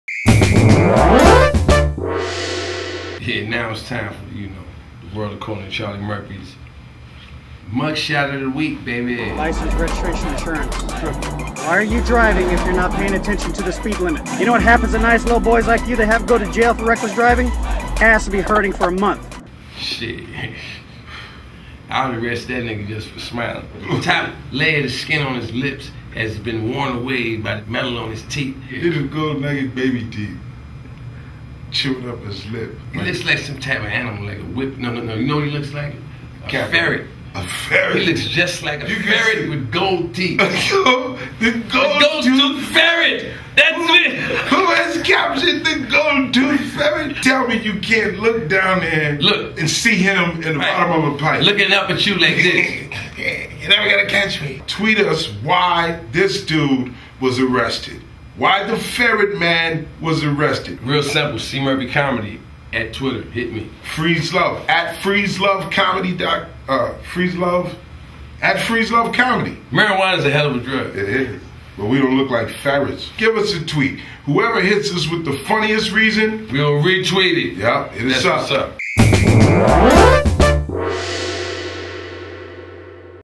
Mugshot Of The Week - Ferret Face
Charlie & Freez deliver an interesting take on this individuals Mugshot